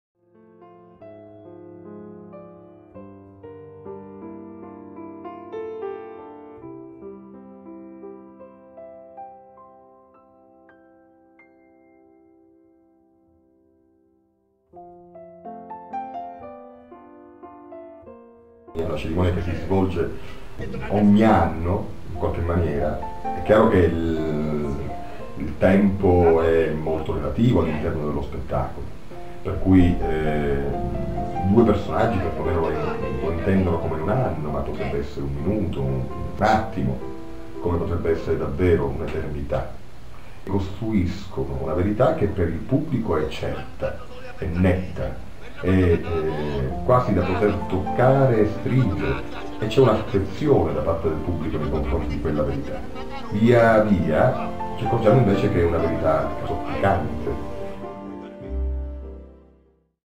Trailer 'A Cirimonia
Due personaggi in scena, ‘U masculu e ‘A fimmina, in un luogo che ha smesso di rivelarsi, celebrano una cerimonia sghemba, che si annuncia inutile. Una cerimonia che li obbliga a un dialogo che solo apparentemente è privo di senso, un dialogo straripante di silenzi pieni di paura che man mano si trasforma, che diviene urlo, esigendo un ritmo da tragedia e una musicalità surreale, in una perfetta idiosincrasia di stati d’animo.
È una cerimonia che si ripete da anni, quella a cui il pubblico assiste. I due personaggi, utilizzando un gioco grottesco (’u Mi ricordu), sono obbligati a ricordare, tentando di appigliarsi a un qualche brandello di verità.
Una verità, però, che inesorabilmente risulterà inafferrabile.
Testo e regia Rosario Palazzolo in scena con Anton Giulio Pandolfo, musiche di Gianluca Misiti, assistente alla regia Angelo Grasso, voci di Alberto Pandolfo e Viola Palazzolo, una coproduzione Circo dell'avvenire e Teatrino Controverso.
Vincitore 18° FIT/Festivale Internazionale di Lugano (Ch) - Menzione speciale InBox 2010